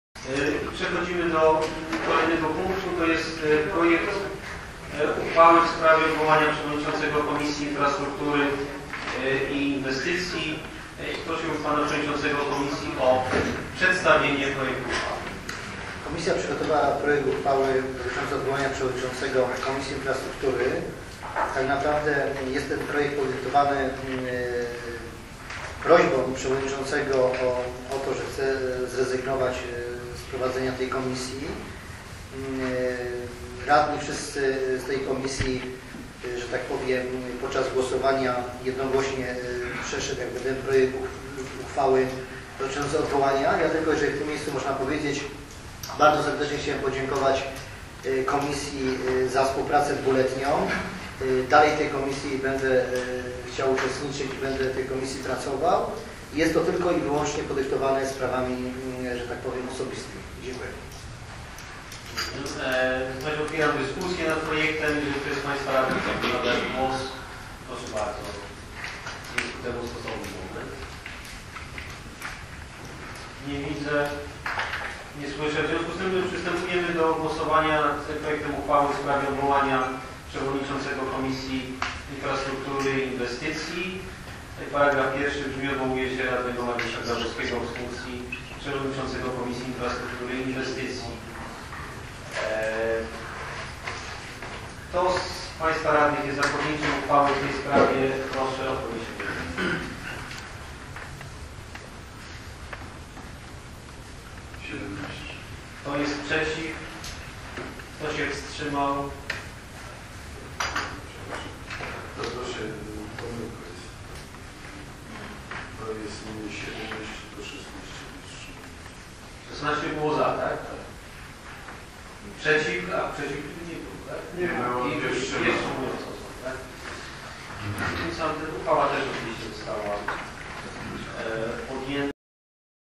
RD_XII_03-01-2012_6 Odwołanie Przewodniczącego Komisji Infrastruktury
Obrady XXVII sesji Rady Dzielnicy XI Podgórze Duchackie z dn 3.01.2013 r.
część 6:
Uchwała w sprawie odwołania Przewodniczącego Komisji Infrastruktury